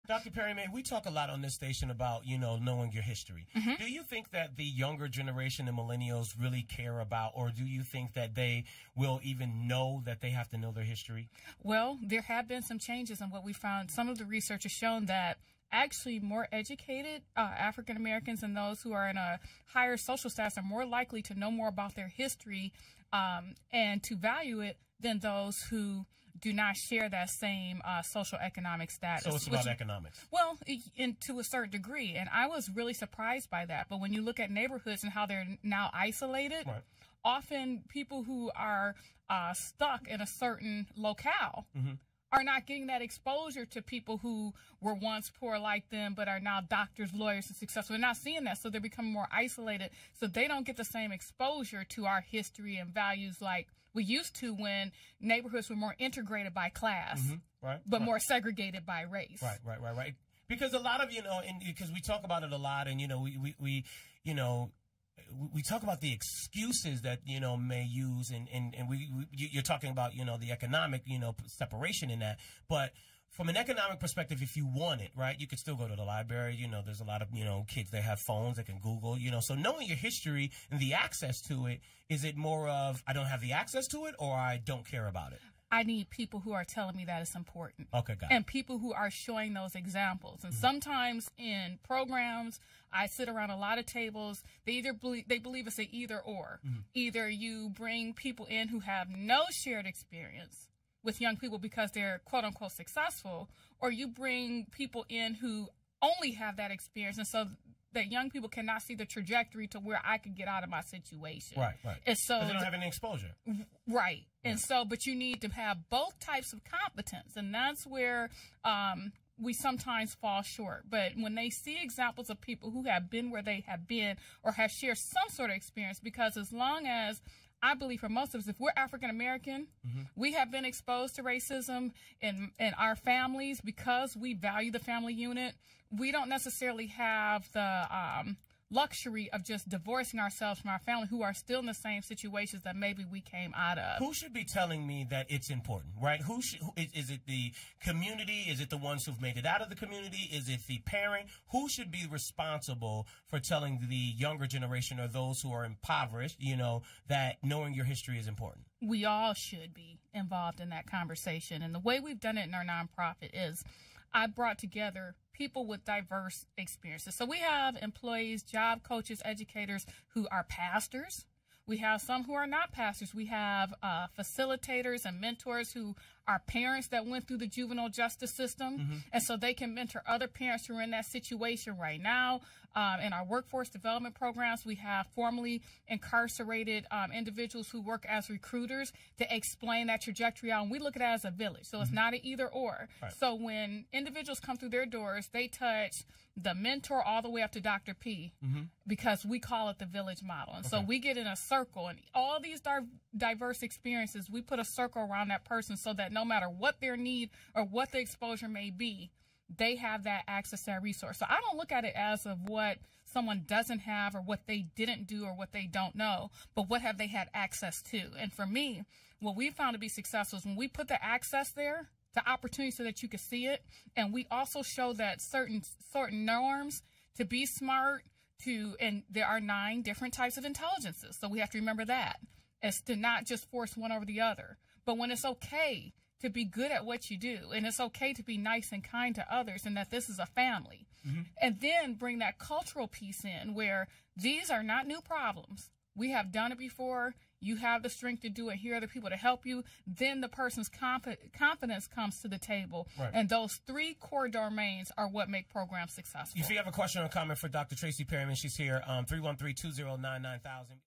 Urban Ministry with Tracee Perryman, PhD. Part Two: Importance of Knowing Your History
Conversation with Tracee Perryman, PhD - executive director of Center of Hope Family Services, Inc. and 910am Superstation host Charles "SPUDD" Spencer